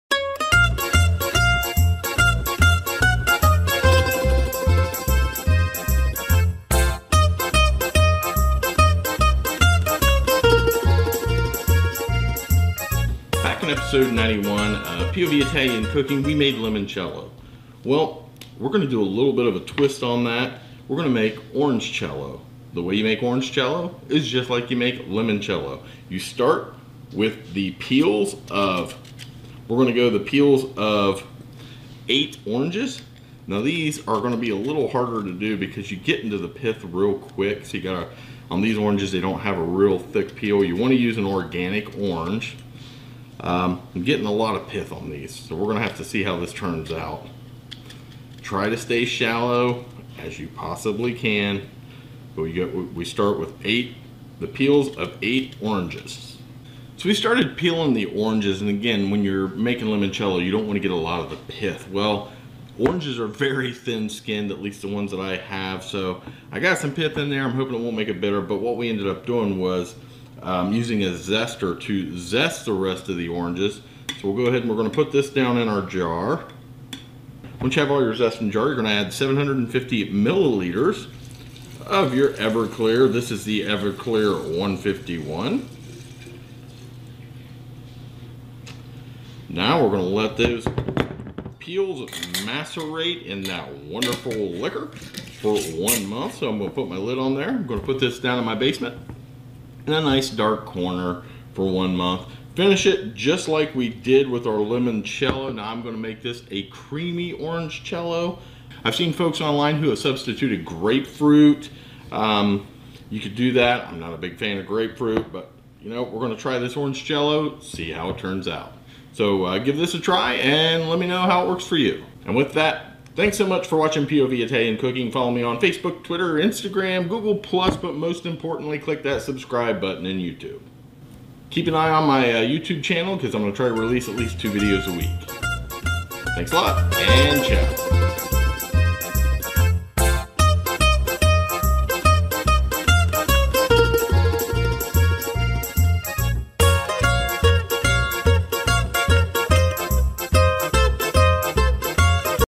Orangecello: POV Italian Cooking Special Episode
In this short video I share how to modify the Limoncello in episode 91 of POV Italian Cooking to make Orangecello.  The Orangecello discussed in this video is for the creamy type.

Watch POV Italian Cooking on the Simply Good Food TV app